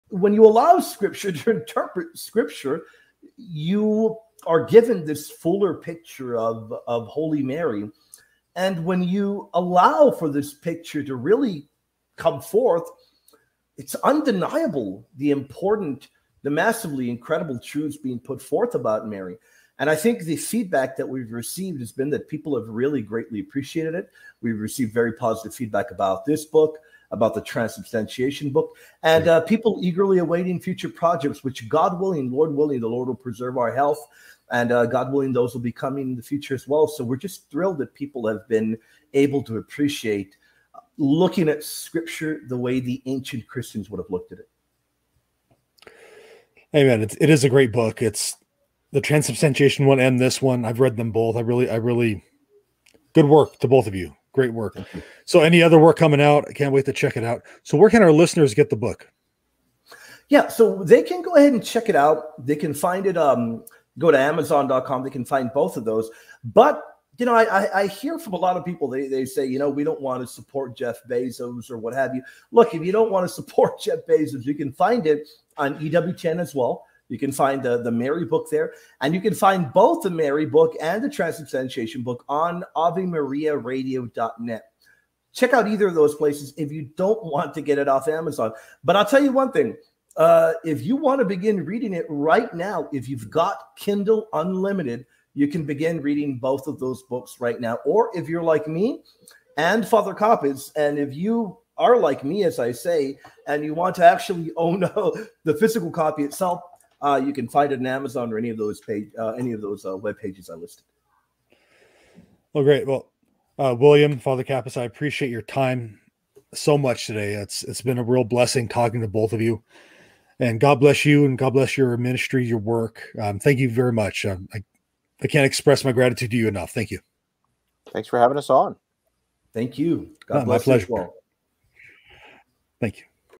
The Hidden Truths About Mary You Need to Know!
Join us as we uncover the hidden truths about Mary that you absolutely need to know! In this captivating discussion, I sit down with William and Father Capas to explore the profound insights revealed in their latest book. By allowing scripture to interpret itself, they paint a fuller picture of holy Mary, unveiling the incredible truths that have deeply resonated with readers. We've received enthusiastic feedback, with many appreciating how these insights reflect the views of ancient Christians.

Whether you prefer a Kindle read or a physical copy, these works are transformative. Join us in this enlightening conversation and discover new revelations about Mary that will deepen your understanding and faith.

Don't miss out—explore these theological treasures today and enrich your spiritual journey. God bless our guests and their incredible work in evangelism and apologetics!

CHAPTERS:
00:00 - Importance of Scripture Interpretation
00:16 - Book Feedback and Review
00:55 - Where to Buy the Book
01:53 - Final Thoughts and Takeaways